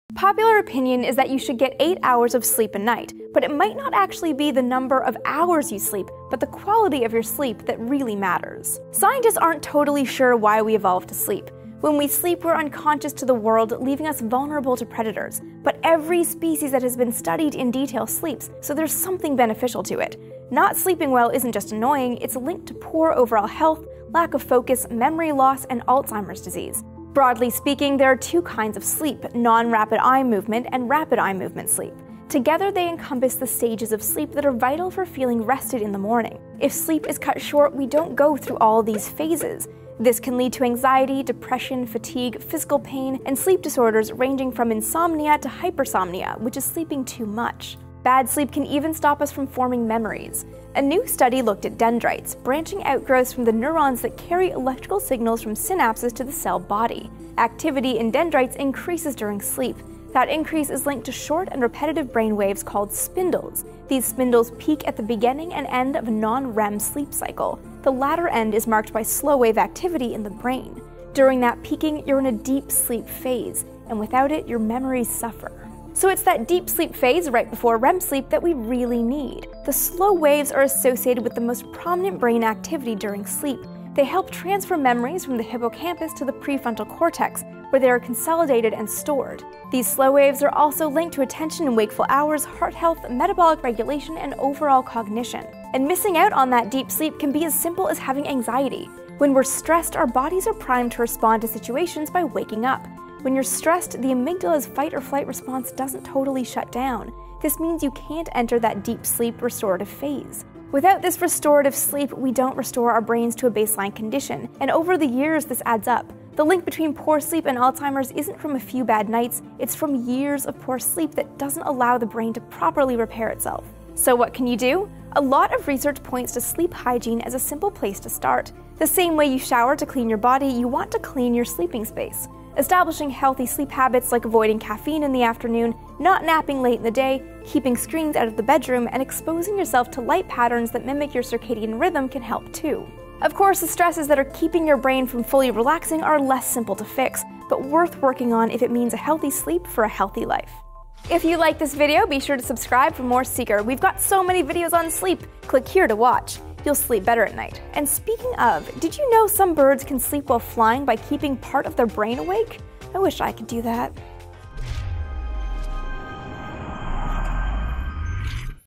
Sleep Can Repair Your Brain, But Only If You’re Doing It Right
Sleep studies have found that quality, not quantity of sleep determines how rested you are. But what is quality sleep and how do we get it?

New insights into how sleep helps the brain to reorganise itself
“A study has given new insights into how sleep contributes to brain plasticity – the ability for our brain to change and reorganise itself – and could pave the way for new ways to help people with learning and memory disorders.”

Snooze Alarm: Sufficient Sleep Improves Human Health and Longevity
“No one likes being tired. You’re less motivated, you’re more irritable, and it’s harder to concentrate. We know getting sleep is important, but until recently, we didn’t understand just how dangerous it is to not get enough. According to some sleep scientists, getting less than 7 hours of sleep per night could mean shortening your life.”

Jellyfish Sleep, and Now Scientists Wonder If Plants and Bacteria Sleep Too
“Despite the fact that jellyfish lack a centralized nervous system, new research provides strong evidence that these soft-bodied marine organisms sleep. The findings, published in the journal Current Biology, have scientists reevaluating what sleep is and what it does.”

Special thanks to Amy Shira Teitel for hosting and writing this episode of Seeker!